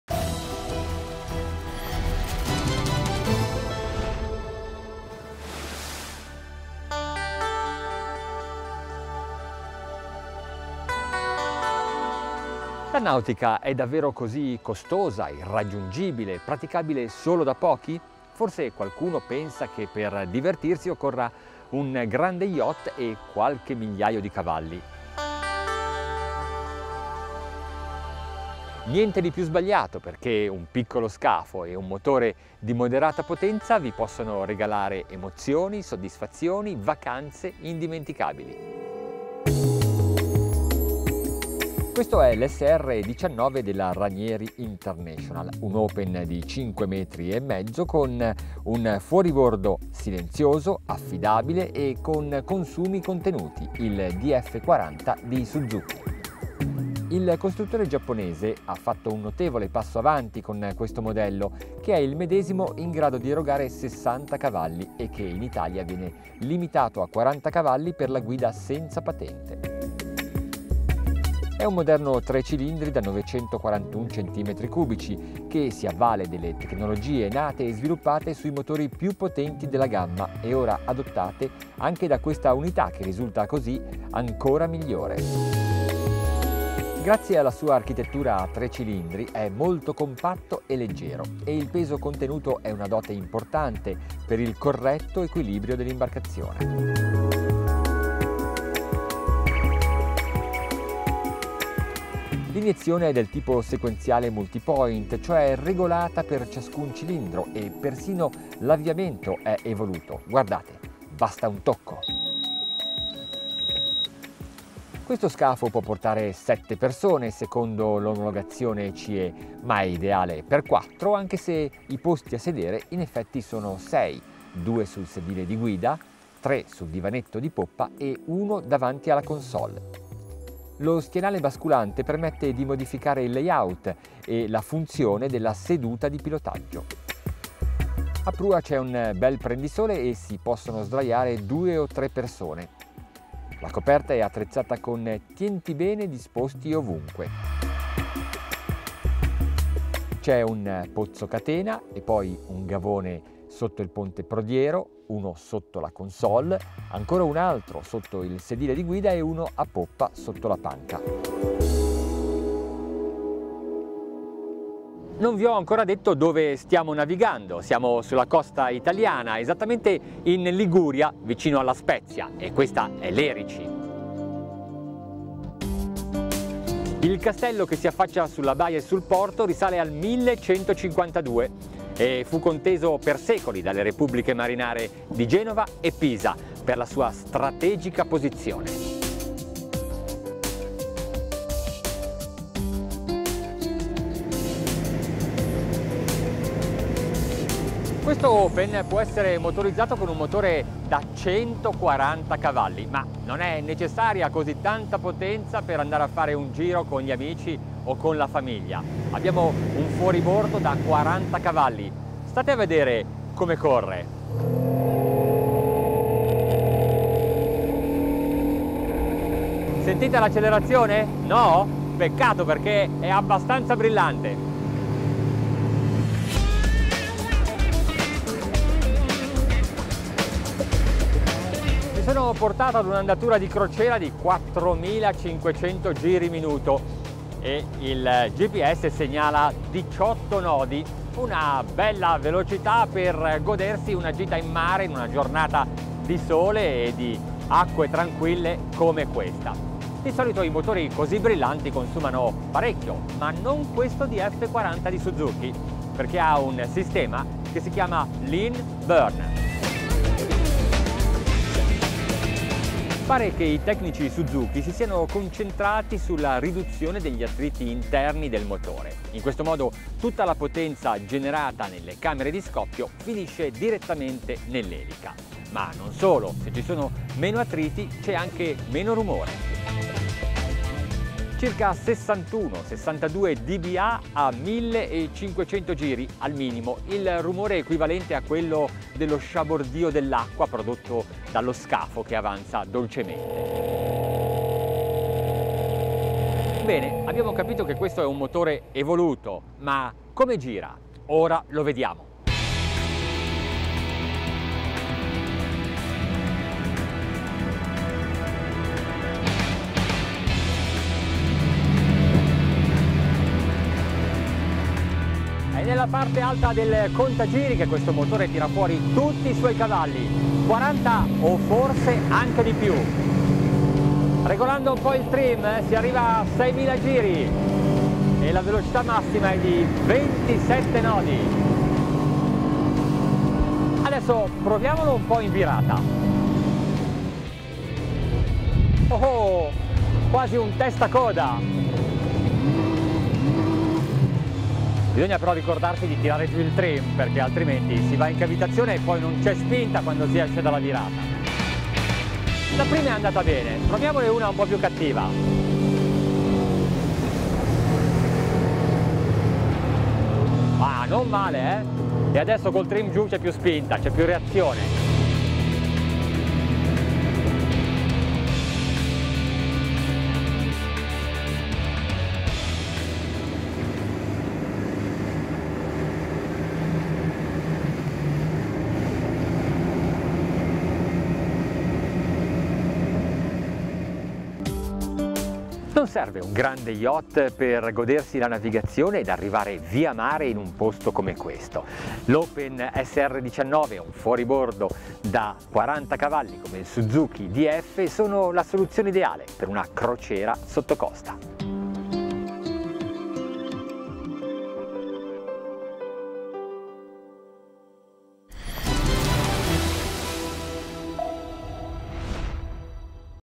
On Board SUZUKI SR19
Prova SUZUKI DF40A e SR19 su The Boat Show con Maurizio Bulleri